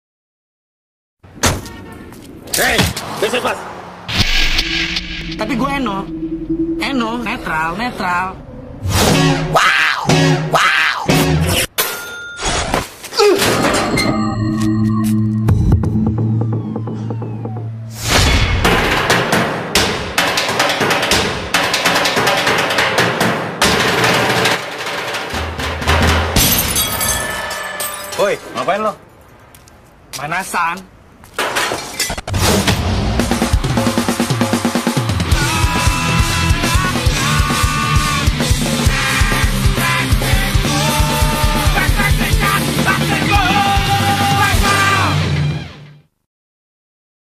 Iklan X Mild - Netral (2007-08) @ RCTI,SCTV,TPI,ANTV,Indosiar,MetroTV,TransTV,Trans7,GlobalTV,Lativi
Iklan ini pernah ditayangkan pd thn 2007-08 di :

- RCTI (Mega Sinetron)
- SCTV (Gala Sinetron)
- TPI (Sinema Asyik)
- ANTV (STAR Sinema)
- Indosiar (Sinetron Unggulan)
- Metro TV (Top Nine News)
- Trans TV (Bioskop Trans TV)
- Trans 7 (Teather 7)
- Global TV (Big Movies)
- Lativi (Kabar Malam)